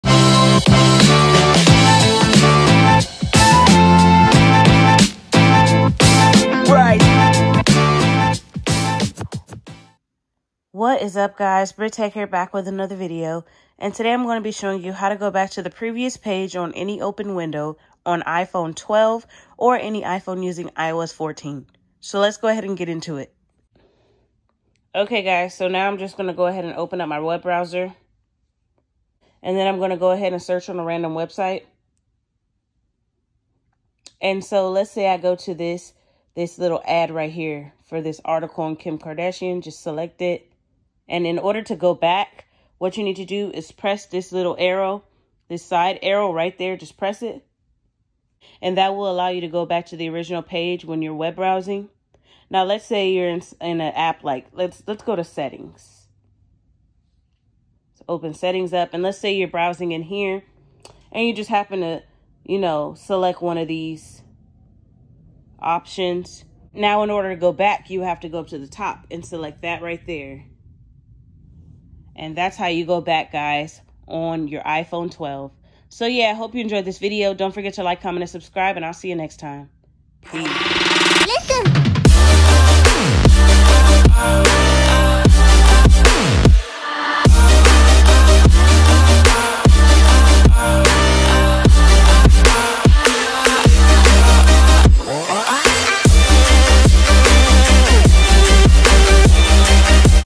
iPhone 12 How To Go Back To Previous Page
In this video I show you how to go back to the previous page in apps on iPhone 12 and iPhone 12 pro.